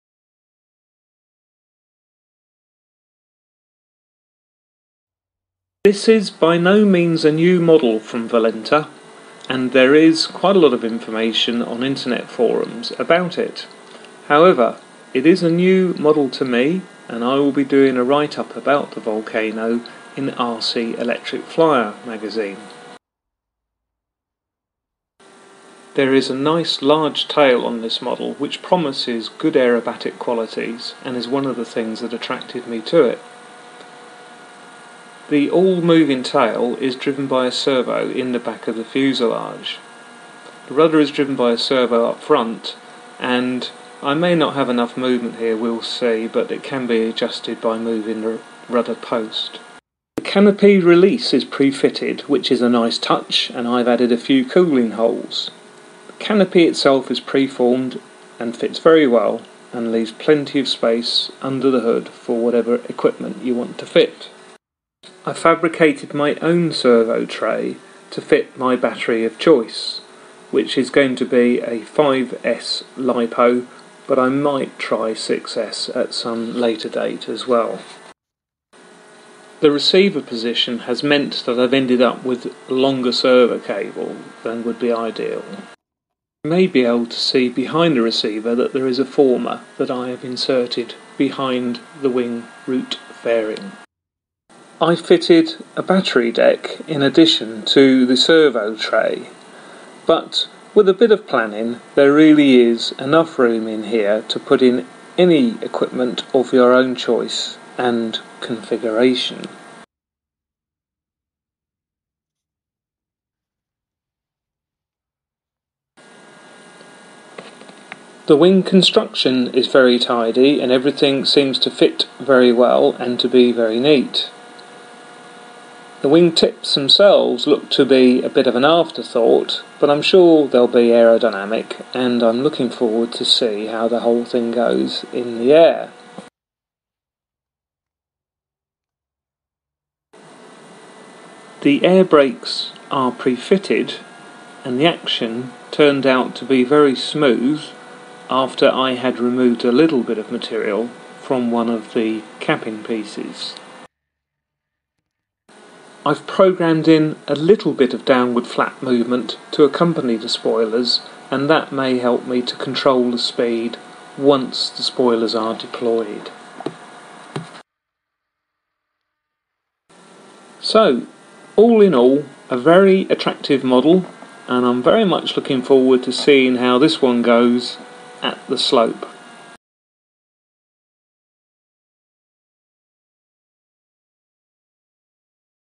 Valenta Volcano - a brief tour
Valenta Volcano Electro 3.7M span
Finished build but not yet flown.  Some details on here to help those considering this model.